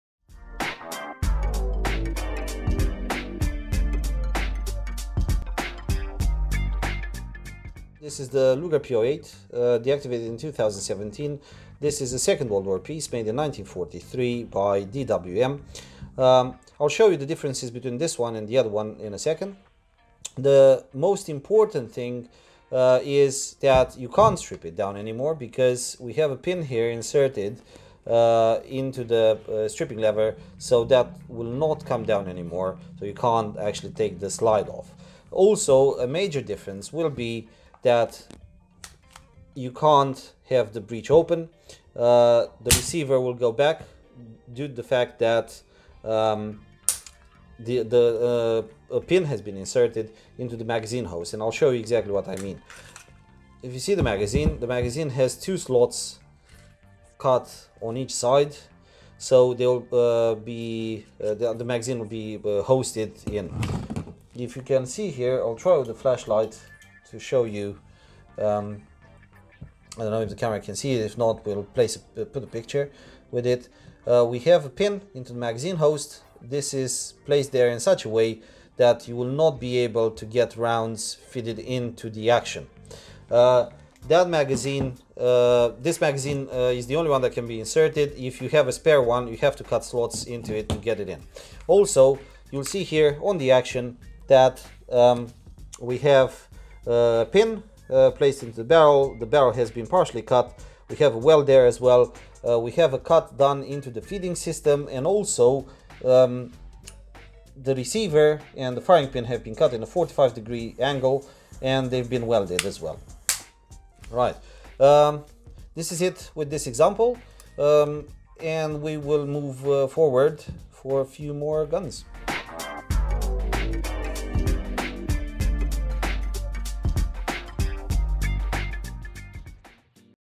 Gun Deactivation: Episode 2. Luger PO8
SOCIAL MEDIA...

Wikipedia quote: The Luger has a toggle-lock action that uses a jointed arm to lock, as opposed to the slide actions of many other semi-automatic pistols. After a round is fired, the barrel and toggle assembly travel roughly 13 mm (0.5 in) rearward due to recoil, both locked together at this point. The toggle strikes a cam built into the frame, causing the knee joint to hinge and the toggle and breech assembly to unlock. The barrel strikes the frame and stops its rearward movement, but the toggle assembly continues moving, bending the knee joint, extracting the spent casing from the chamber, and ejecting it. The toggle and breech assembly then travel forward under spring tension and the next round is loaded from the magazine into the chamber. The entire sequence occurs in a fraction of a second and contributes to the mud-resistance[9] of the pistol.

This mechanism works well for higher-pressure cartridges, but cartridges loaded to a lower pressure can cause the pistol to malfunction because they do not generate enough recoil to work the action fully. This results in the breech block either not clearing the top cartridge of the magazine or becoming jammed open on the cartridge's base.[10] This malfunction with under-powered cartridges does occur with Browning-type and other pistol designs as well, but the Luger is sensitive to cartridges other than the brass-cased ammunition that it was designed to use.[11]

Sub-machine guns were found to be effective in trench warfare during World War I, and experiments were conducted to convert various types of pistols to fully automatic machine pistols, including the P08. The Luger proved to have an excessive rate of fire in full-automatic mode, however, as did the Mauser C96.